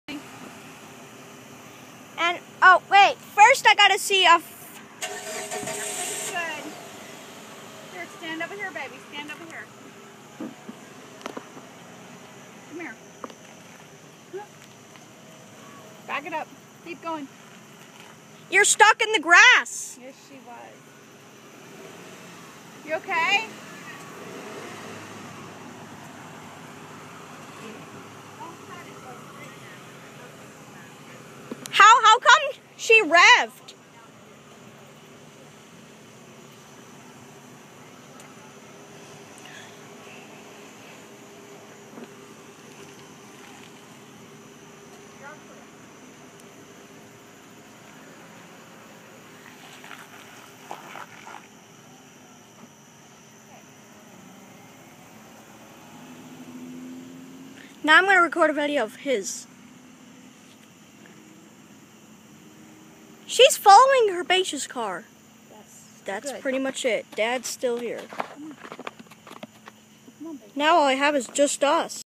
Take off with my babysitters 2006 ford escape and my ex boy
My ex boy friends car follows my babysitters car. LOL.